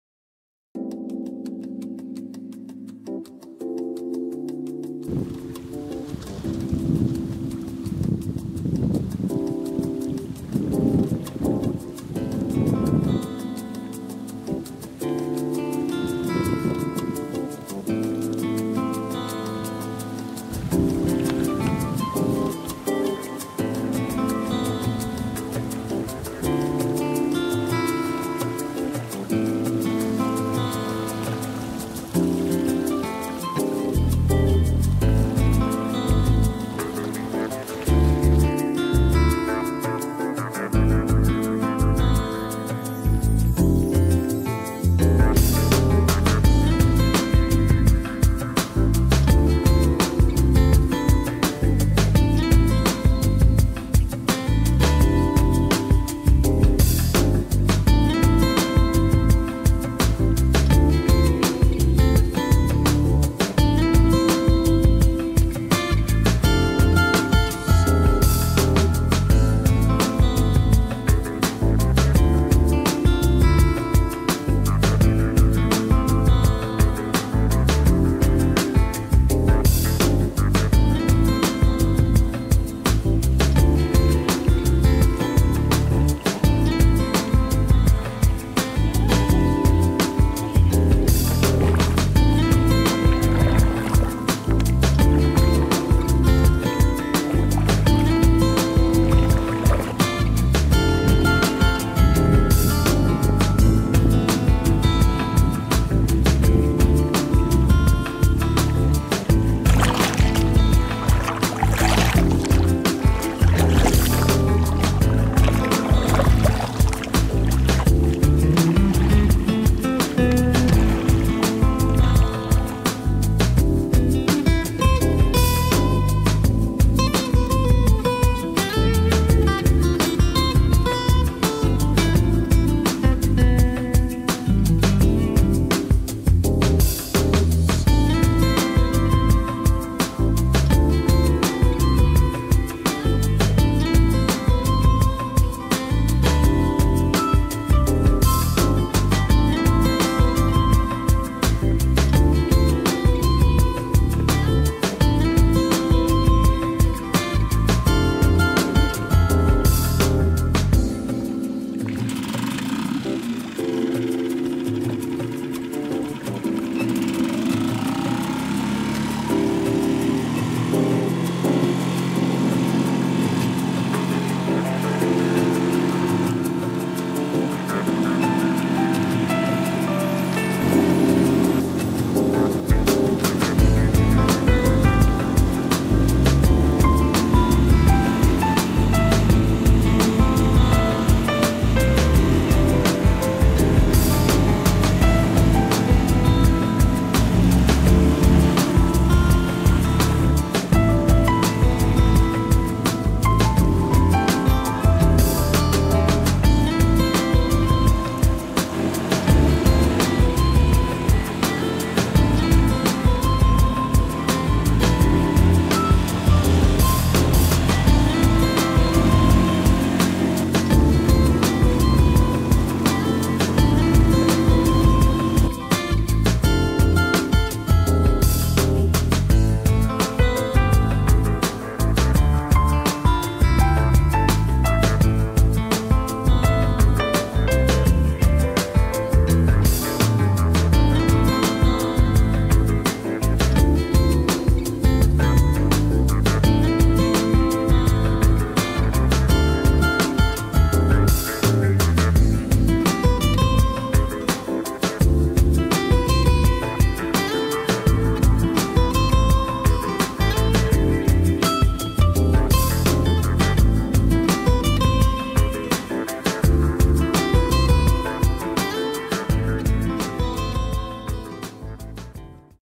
GRAND inflatable canoe ARGUS A380-Discovery paddling and driving with 3.5HP motor
GRAND Inflatable canoe ARGUS A380 is the smallest model from ARGUS range : A380, A450, A550.  This canoe is rated for 3 adults, and is a really perfect unit for 2 people recreational paddling and runabouts.  With two large fins in the rear it demonstrated exceptional directional stability.  3.5HP motor is pushing the canoe with 2 people up to 15-20 km/h.  With very small package size it becomes unbeatable tool for smart canoeing!